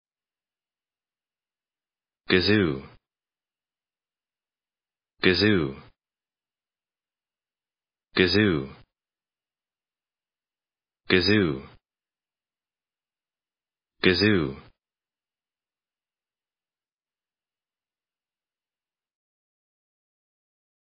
How to pronounce gazoo
gazoo American English pronunciation. How to pronounce gazoo correctly. How to say gazoo in proper American English.